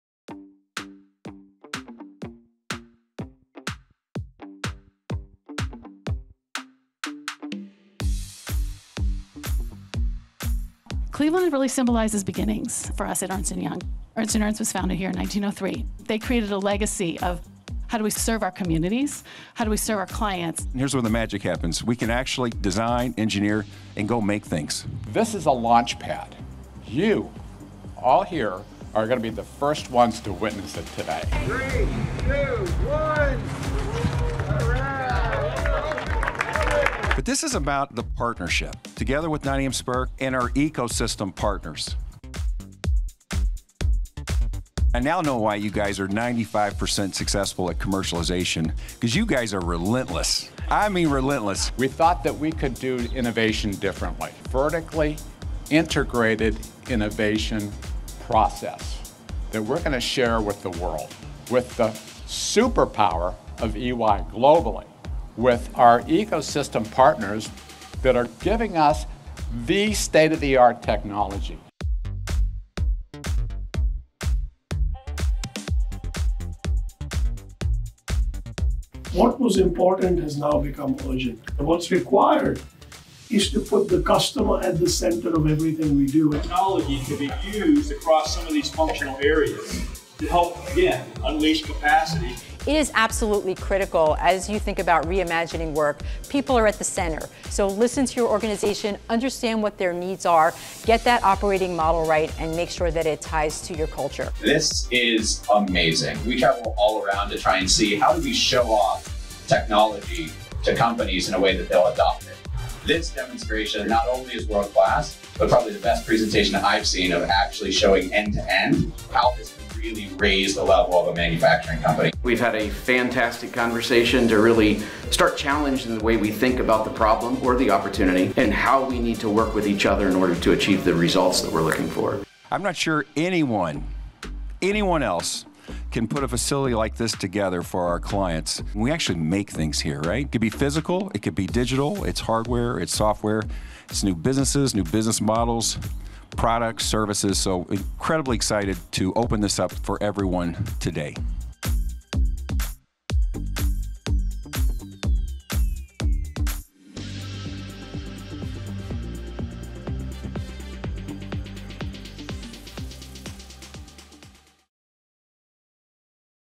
EY- Nottingham Spirk Innovation Hub: grand opening
View highlights from the EY-Nottingham Spirk Innovation Hub grand opening. To learn how you can supercharge growth at the Hub,

In October 2021, EY and Nottingham Spirk announced the opening of the EY-Nottingham Spirk Innovation Hub in Cleveland, OH. The Hub, which has been supporting clients virtually throughout the pandemic, combines Nottingham Spirk’s design, engineering and product development offerings with the global EY organization’s strategy and commercialization capabilities and wavespace network.